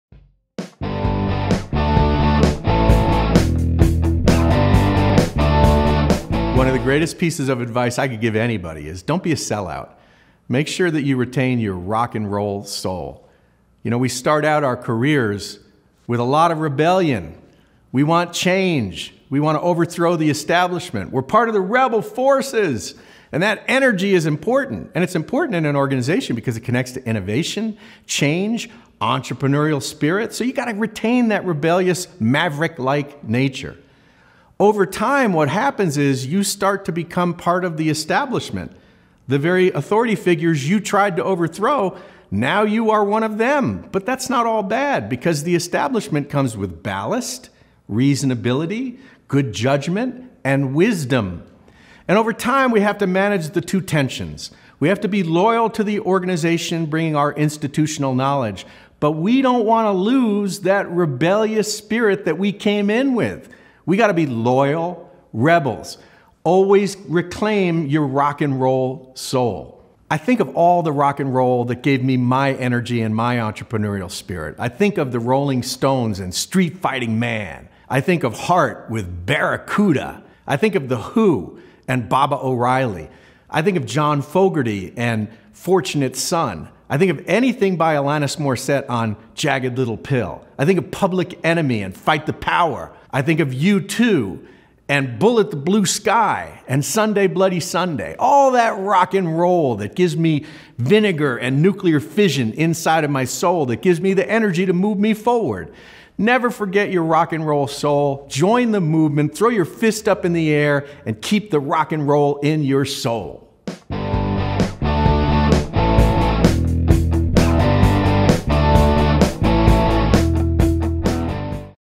Don't Sell Your Rock 'n Roll Soul
We start out our careers with a lot of rebellion — we want to overthrow the establishment. But as time goes on, we may lose some of this entrepreneurial spirit. Bill says loud and proud, "Don't sell your rock 'n roll soul!"